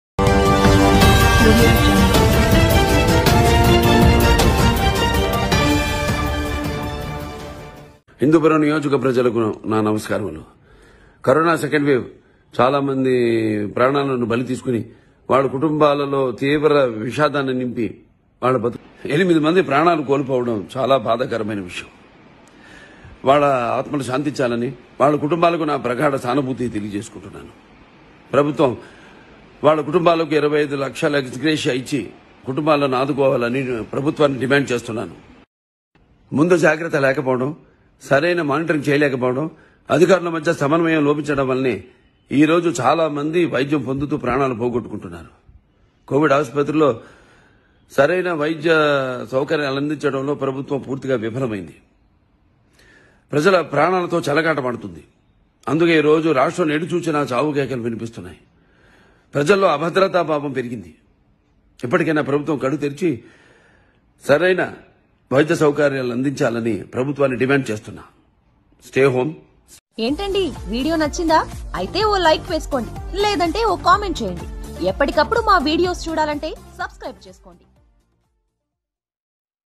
ఒక్కొక్కరికి 25 లక్షలు ఇవ్వాలని జగన్ ని డిమాండ్ చేసిన బాలకృష్ణ | Fata Fut News
Watch#